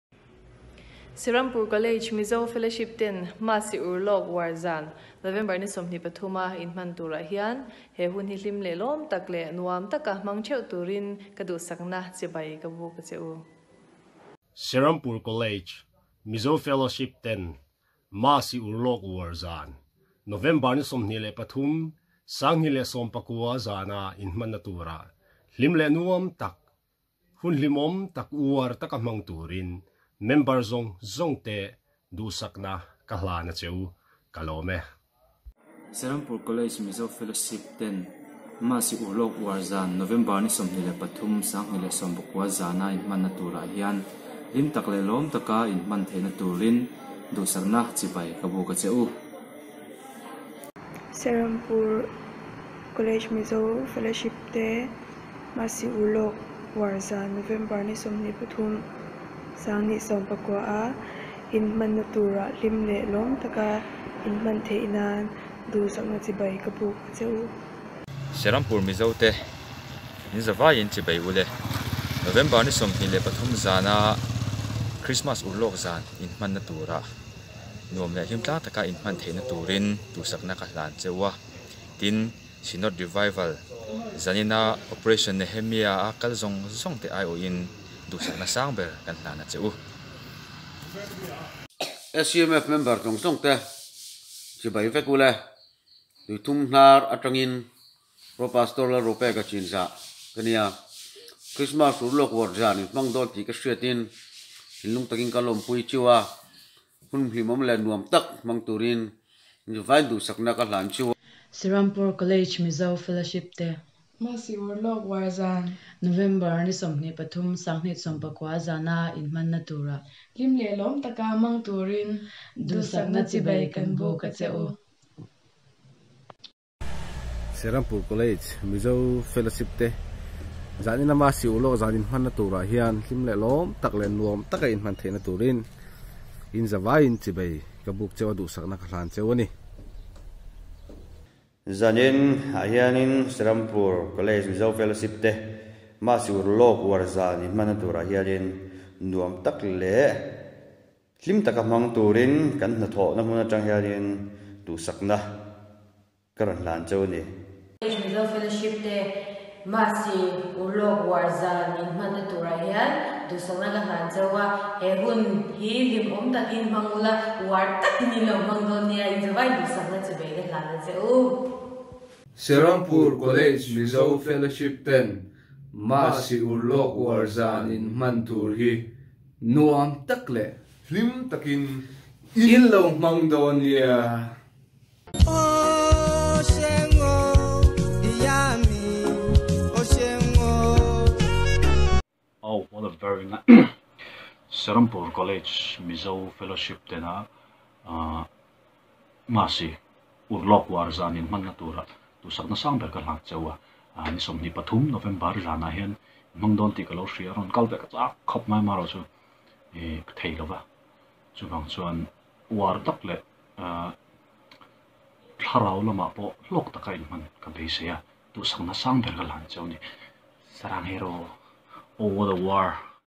CHIBAI BUKNA | SCMF MASI URLAWK UAR ZAN| 23rd Nov. 2019 (Greetings)
Serampore College Mizo Fellowship (SCMF) MASI URLAWK UAR ZAN (Pre-Christmas Celebration) Chibai Bukna
Thanks to Bethsy Lalrinsangi (Mizo Gospel Singer, Lunglei, Mizoram), John Chhana (Mizo Youtuber, Shillong, Meghalaya) and all the Serampore College Mizo Fellowship Ex-Members for your wishes.
May God bless you all for making our programme successful.

Videos Collected by Ms. Lallawmsangi & Edited by Lawrence S. Khawzawl.